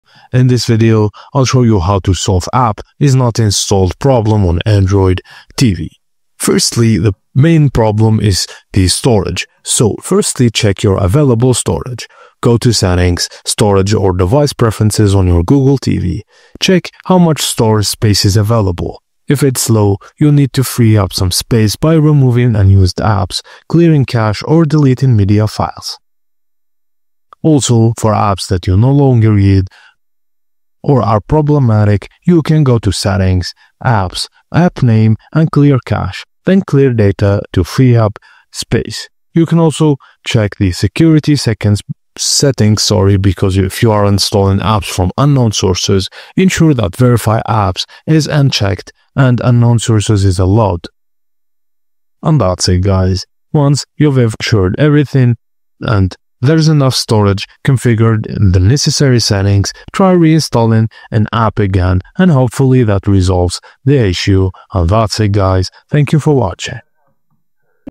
How to Solve APP Not Installed Problem on Android TV❗(2025) (Step-by-Step Guide)✅
🎥 In this video I'll show you Fix App Not Installed Android TV. The method is very simple and clearly described in the video. 🍳 Follow all of the steps & Fix App Not Installed Android TV

00:00.Intro
00:05.Fix App Not Installed on Android TV
00:40.outro